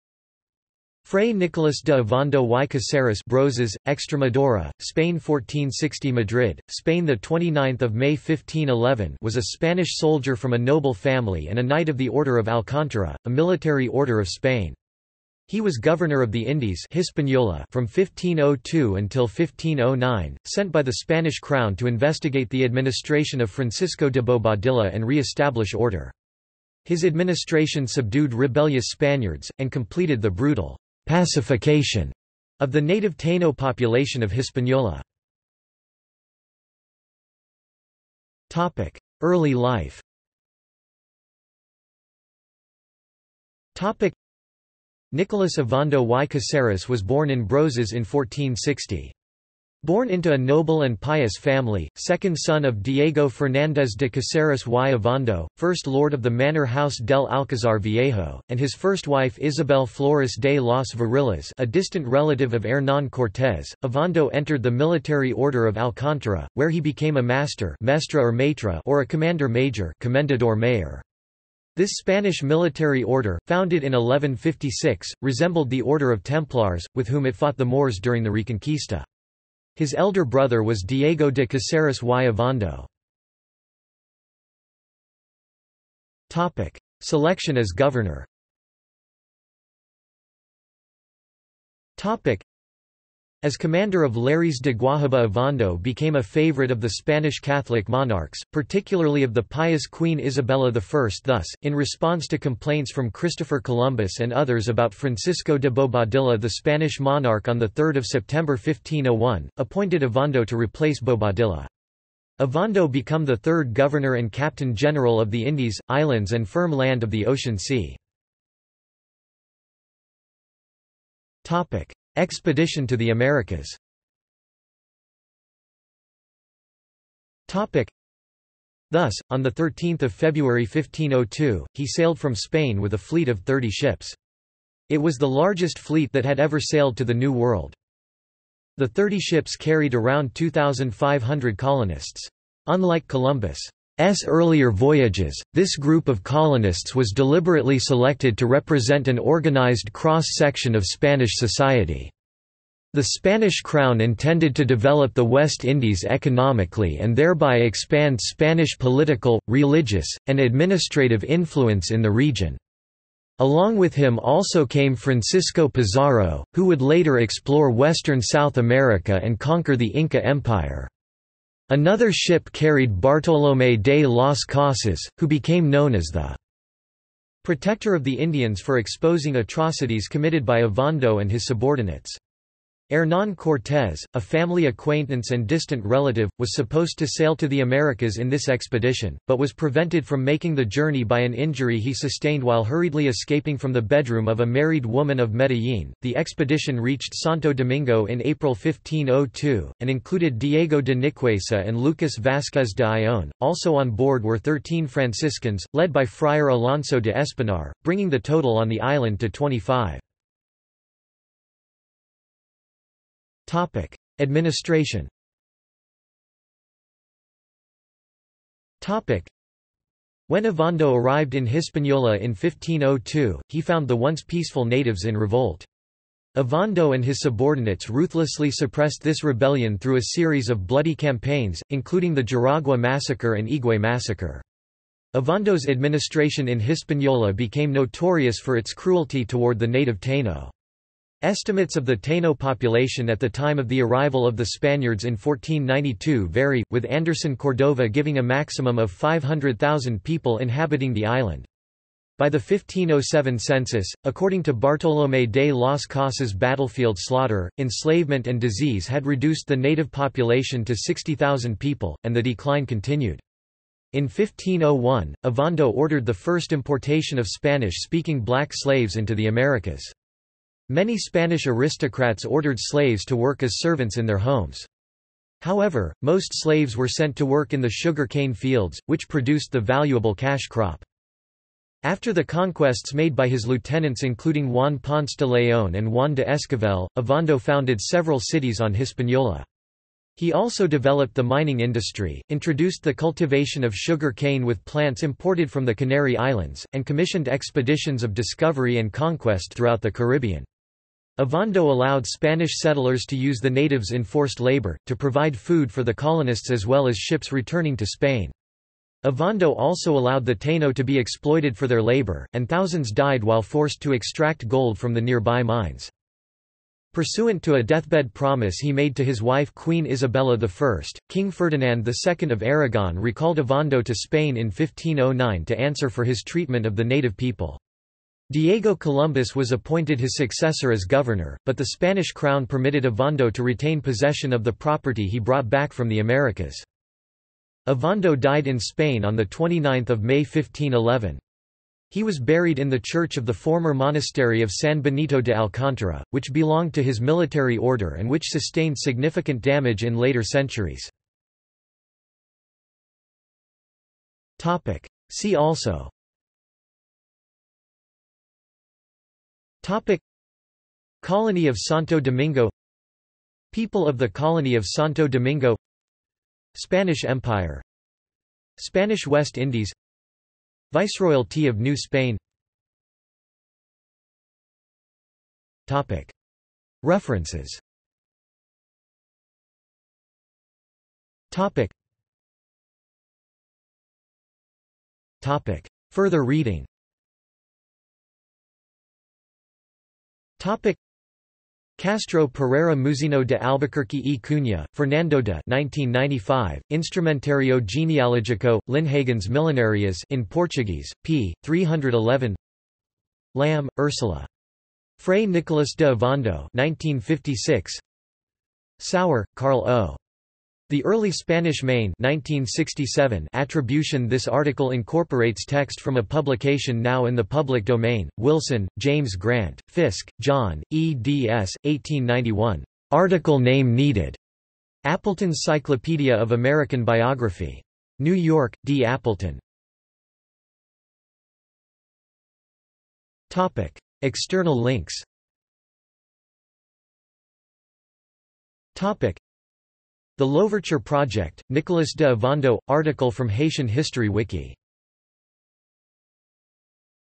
Nicolás de Ovando | Wikipedia audio article
This is an audio version of the Wikipedia Article:
Nicolás de Ovando

SUMMARY
Frey Nicolás de Ovando y Cáceres (Brozas, Extremadura, Spain 1460 – Madrid, Spain 29 May 1511) was a Spanish soldier from a noble family and a Knight of the Order of Alcántara, a military order of Spain. He was Governor of the Indies (Hispaniola) from 1502 until 1509, sent by the Spanish crown to investigate the administration of Francisco de Bobadilla and re-establish order. His administration subdued rebellious Spaniards, and completed the brutal "pacification" of the native Taíno population of Hispaniola.